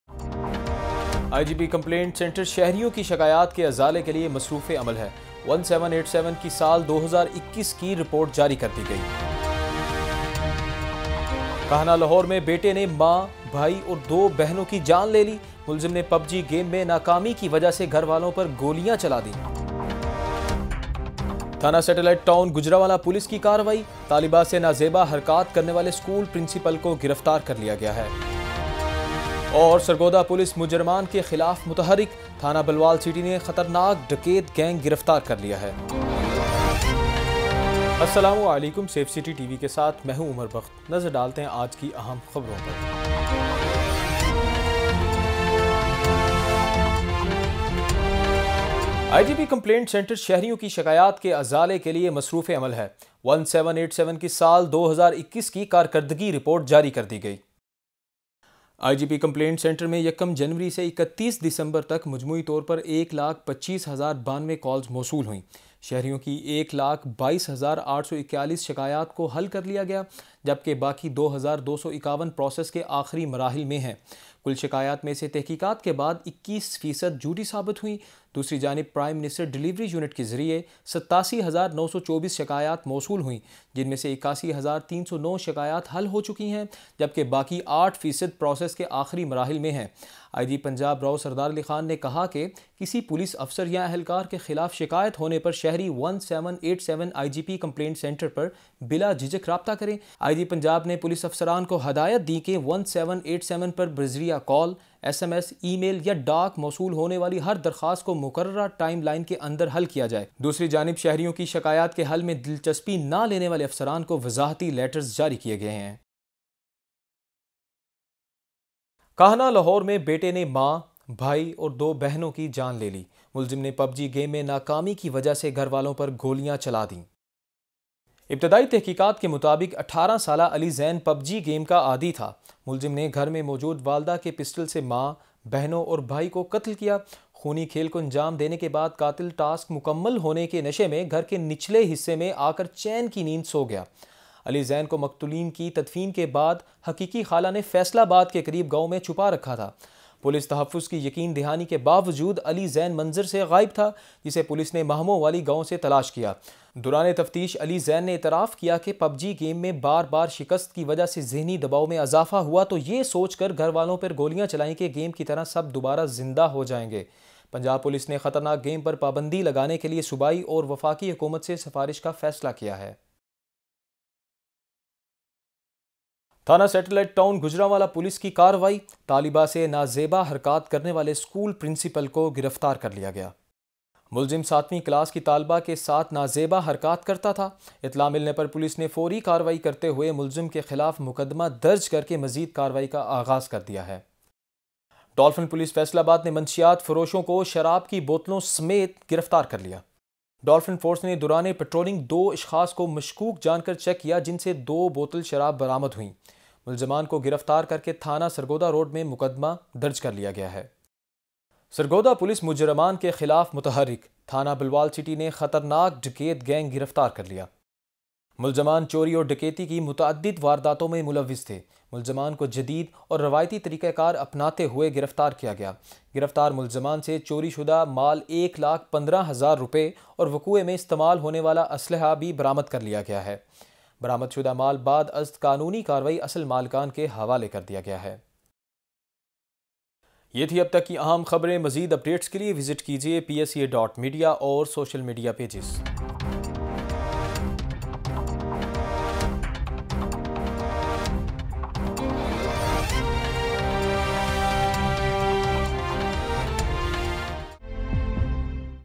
Son killed his mother, brother and two sisters due to failure in playing PubG game | 28 Jan 2022
Son killed his mother, brother and two sisters due to failure in playing PubG game | 28 Jan 2022. Watch Punjab Police and PSCA TV NEWS